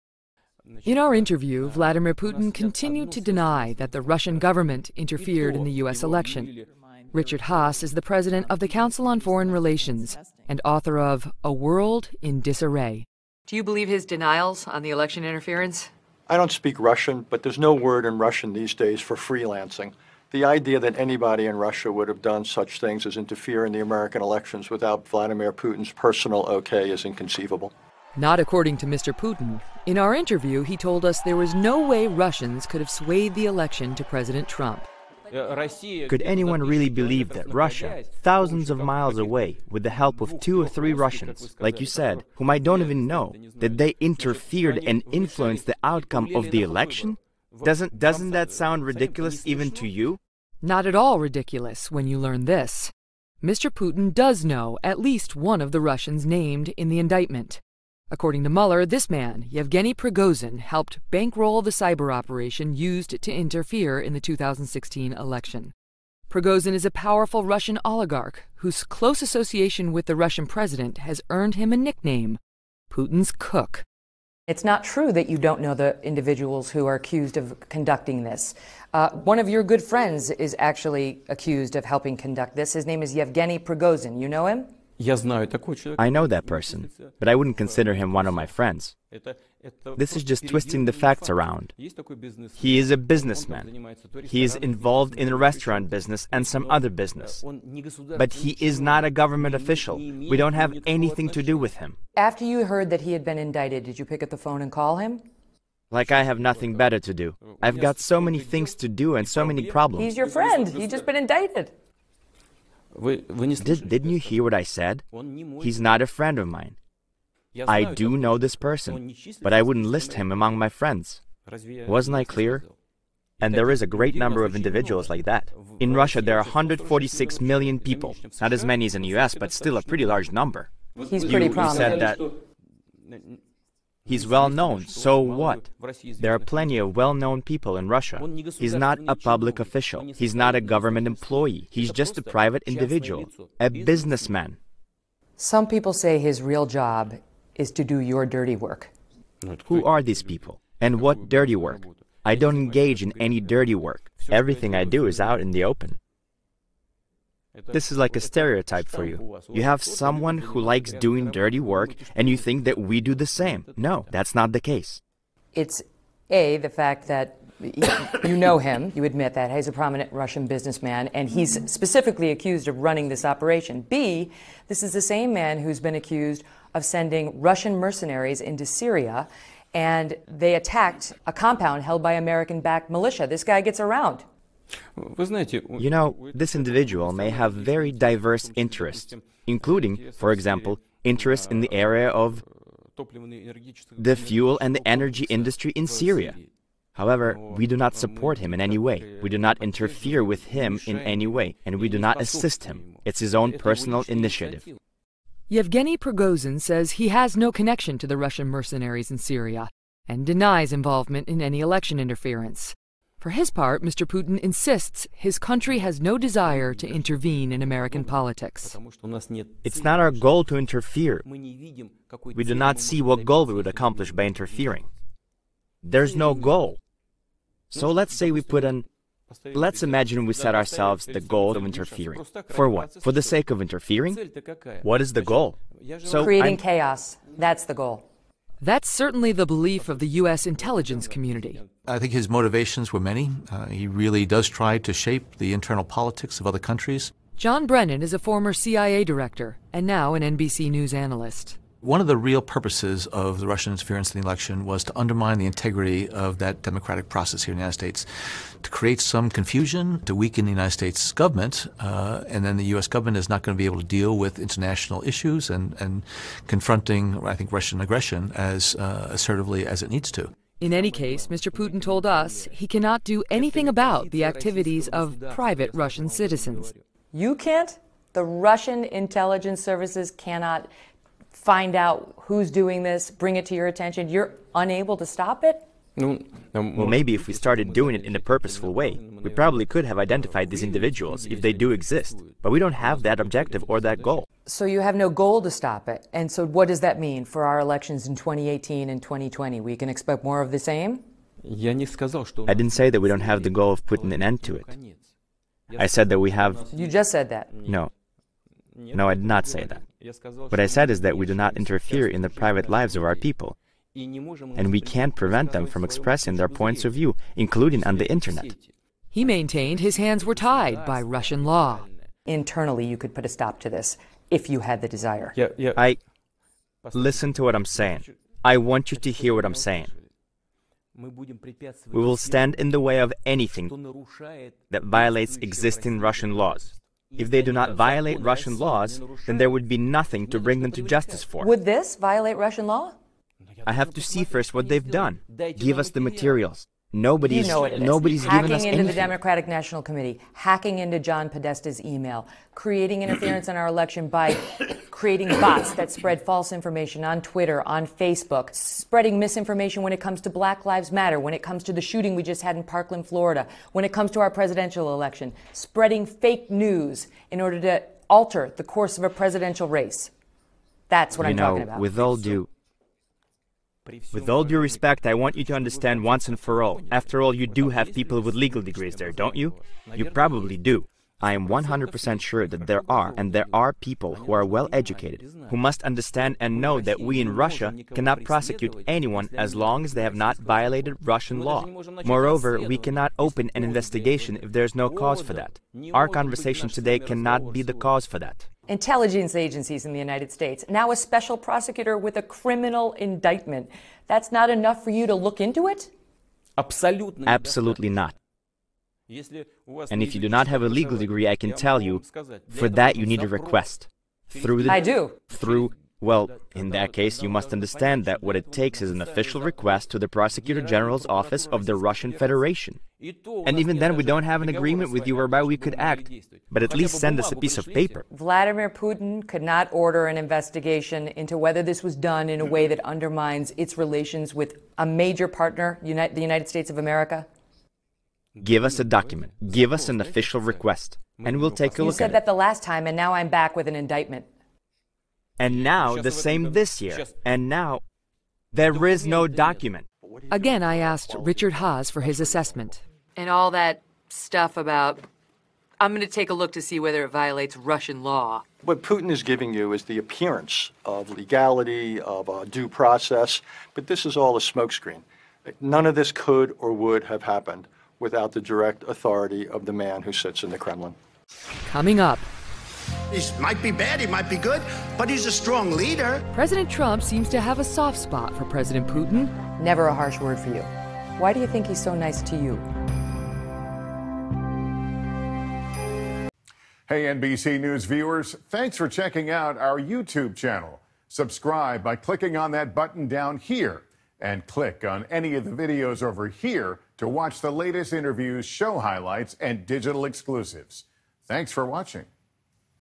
Confronting Russian President Vladimir Putin, Part 2 | Megyn Kelly | NBC News
Megyn Kelly drills down on President Putin's denial of his role in election interference, equipped with allegations detailed in Robert Mueller’s indictment of 13 Russian citizens.